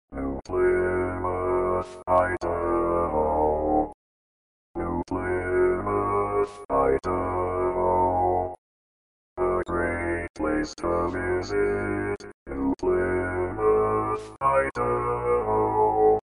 How to Say or Pronounce USA Cities — New Plymouth, Idaho
This video shows you how to say or pronounce New Plymouth, Idaho.

A computer said New Plymouth, Idaho. How would you say New Plymouth, Idaho?